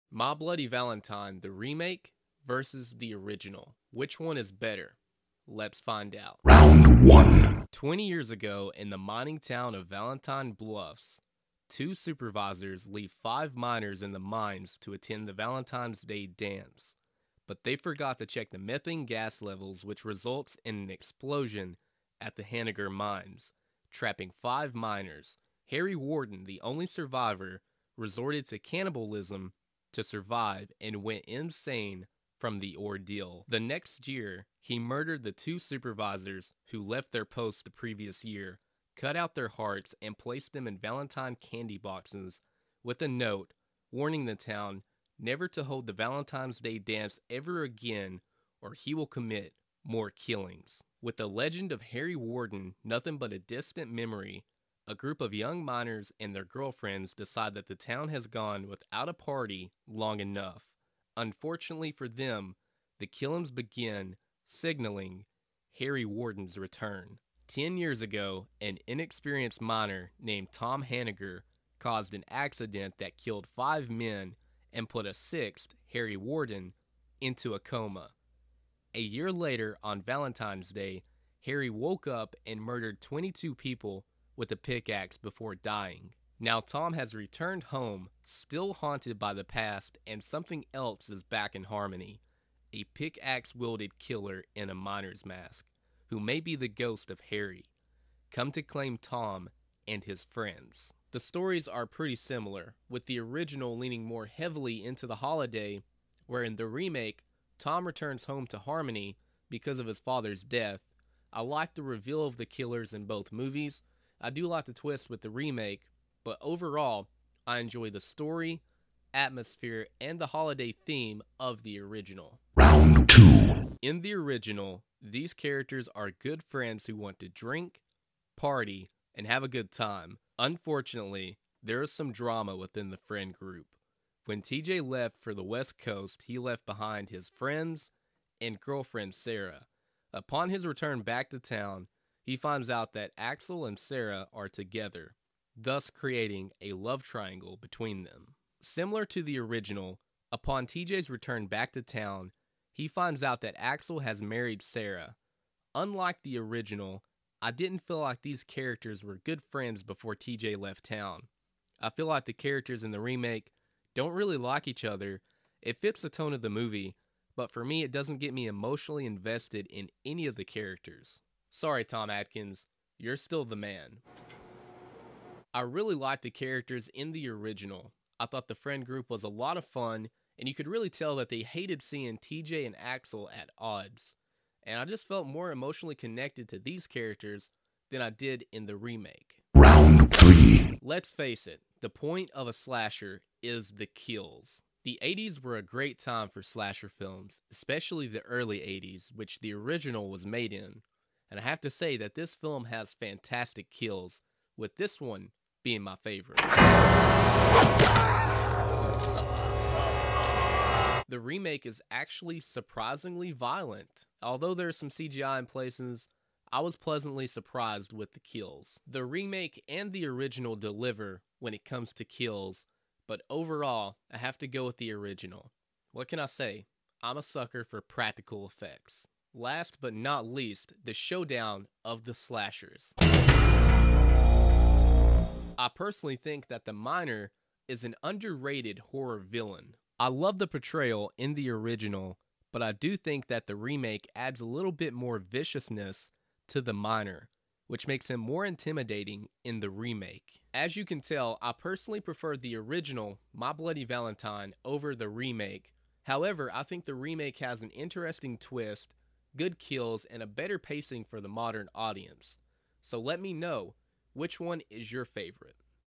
My Bloody Valentine Original Vs Remake
In this video I’ll be comparing the Original 1981 My Bloody Valentine against the 2009 Remake to see which film is better!

Chapters:
0:06 Story
2:02 Characters
3:09 Kills
3:53 The Miner
4:11 Final Thoughts